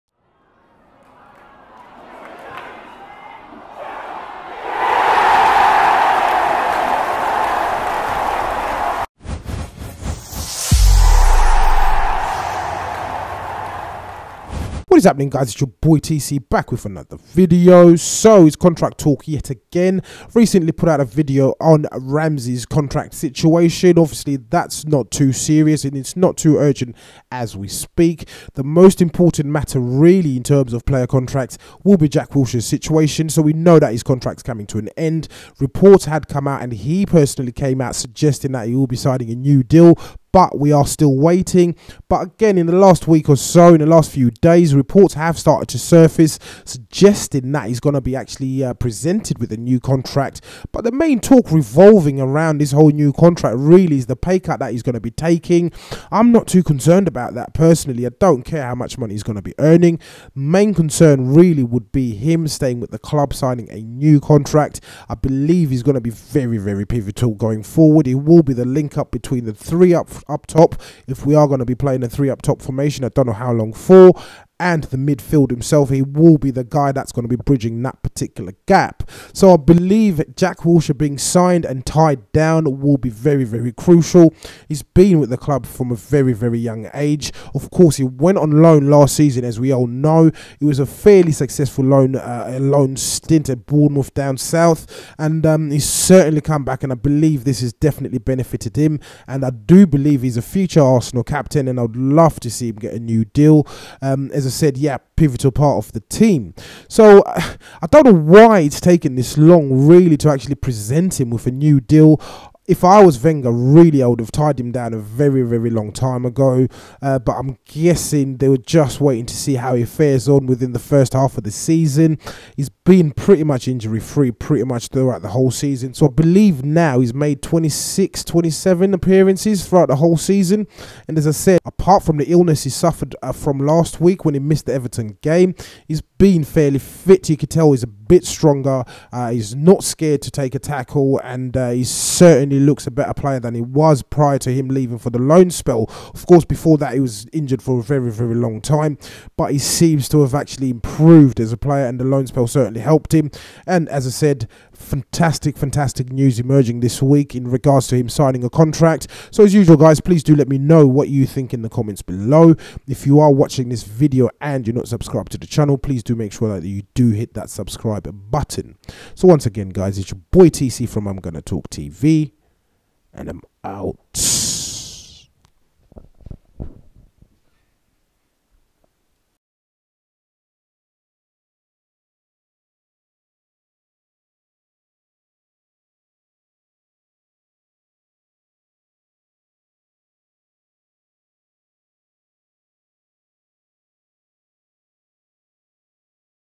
Jack Wilshere Close To Signing New Arsenal Contract
Arsenal have formally offered Jack Wilshere a new contract - which includes a hefty pay cut.

The resurgent midfielder would be offered a new deal on significantly reduced terms.

Wilshere's current basic wage is in excess of £90,000 per week plus lucrative bonuses that can take his earnings to £120,000 per week.

But Arsenal have offered Wilshere a basic wage of £80,000 per week. The club have also offered Wilshere a lucrative bonus package to help boost his deal.

Wilshere is desperate to stay at Arsenal but the new offer could prove a stumbling block in the midfielder committing his future to the Gunners.